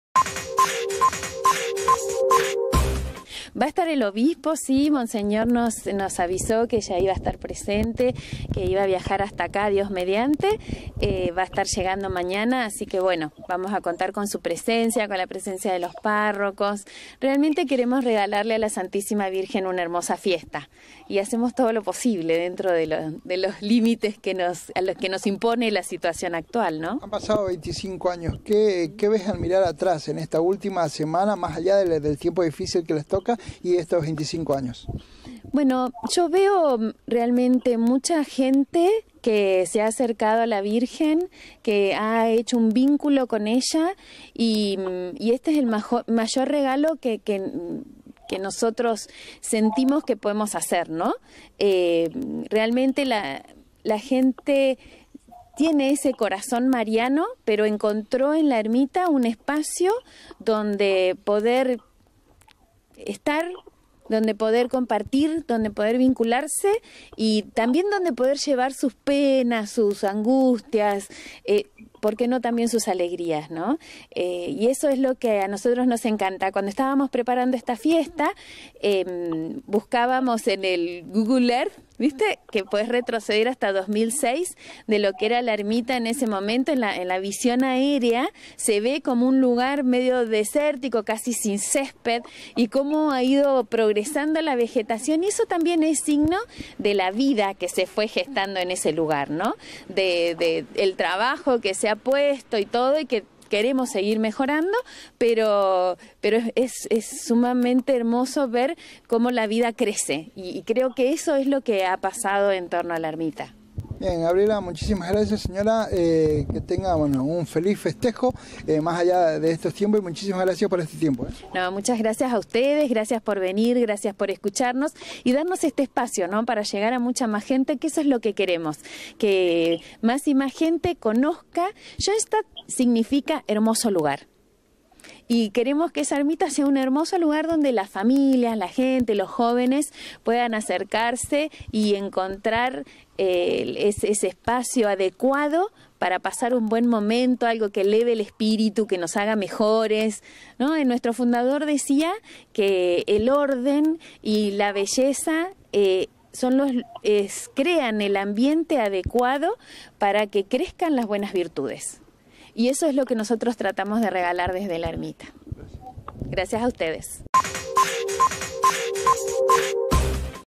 GABRIELA HEINECKE INIVERSARIO ERMITA DE SCHOENSTATT 2
Una de las referentes de la familia Schoenstattiana habló con la prensa e invitó a participar de la santa Misa para celebar los 25 años de la ermita